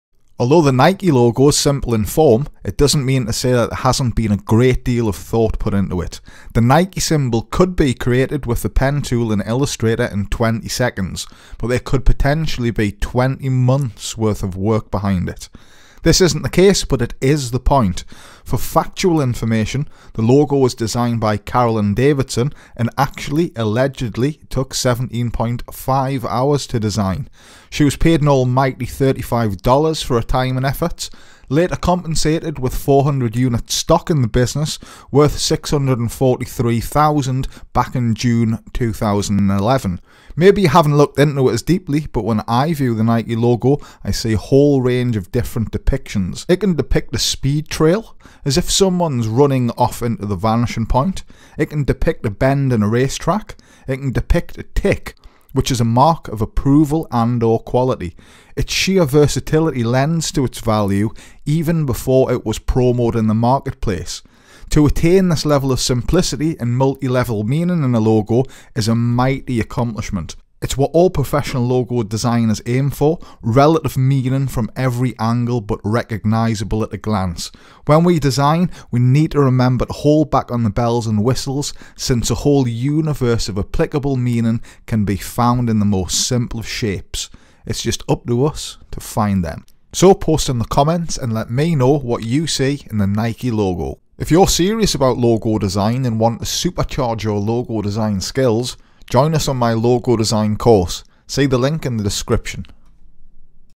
The Madness of the NIKE Logo Design
A review of the Nike logo.